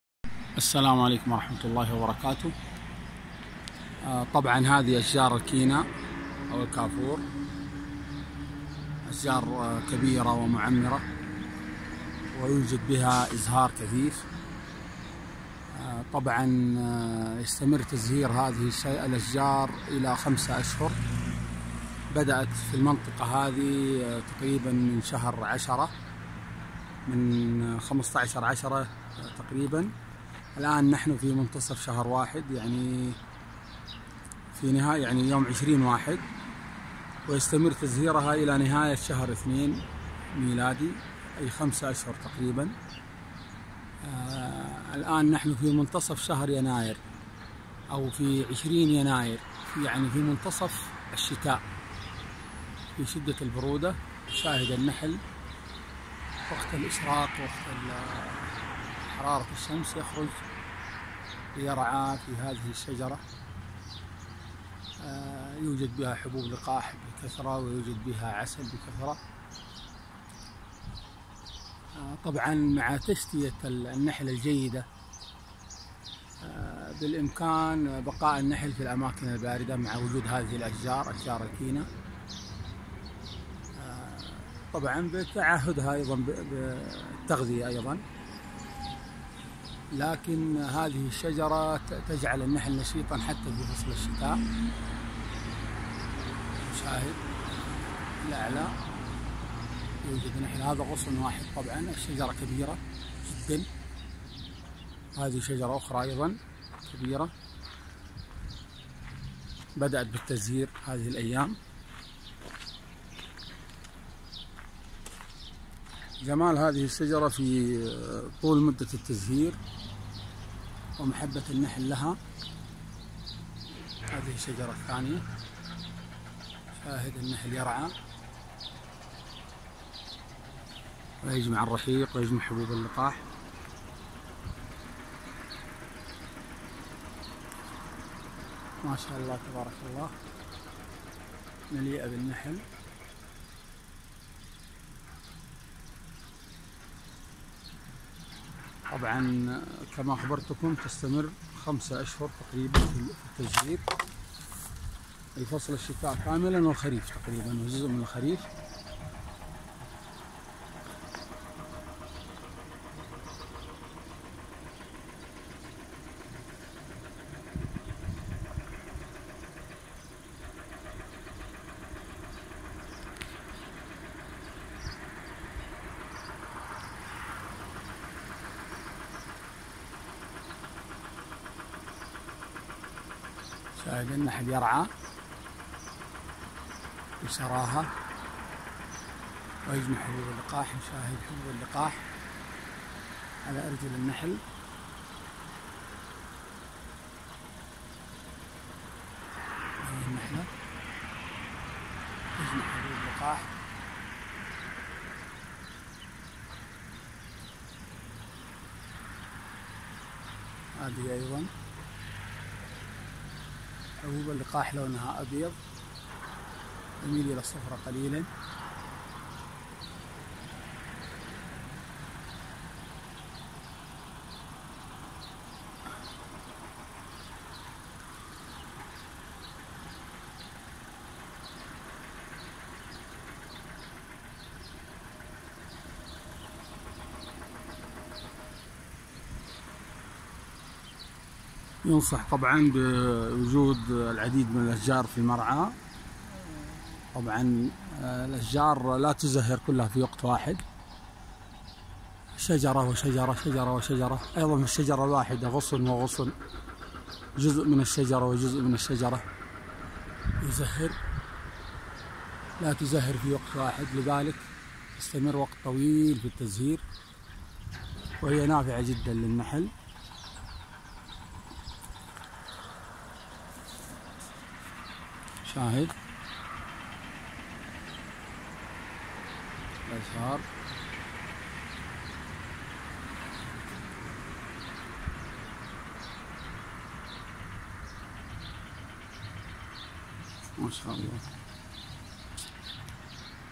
beekeeping أشجار الكينا ( الكافور) أفضل مرعى شتائي للنحل 20-1-2019
منحل العصري -beekeeping الطائف... أشجار الكينا ( الكافور) أفضل مرعى شتائي للنحل  بحيث يستمر تزهيرها لما يقرب من ٥ أشهر في فصلي الشتاء والخريف